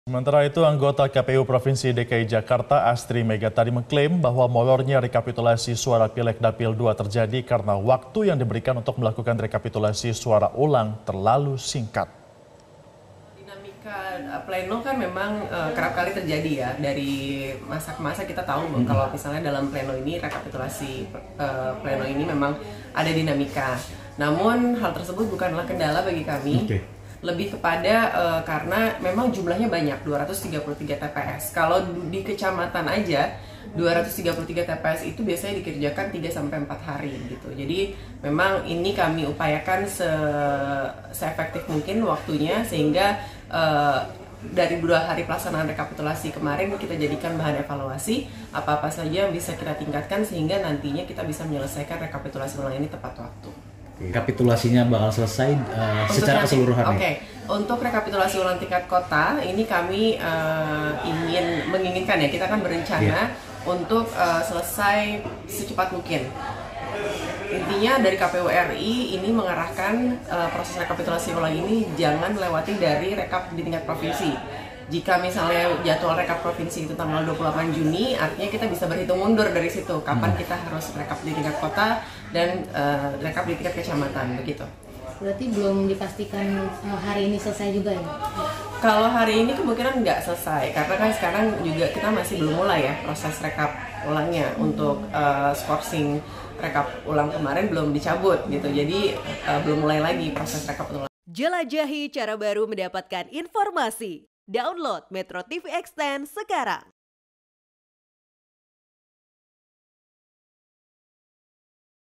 KPU Jakut Keluhkan Waktu Rekap yang Terlalu Singkat
MetroTV, Anggota KPU Provinsi DKI Jakarta Astri Megatari mengklaim bahwa molornya rekapitulasi suara pileg Dapil 2 terjadi karena waktu yang diberikan untuk melakukan rekapitulasi suara ulang terlalu singkat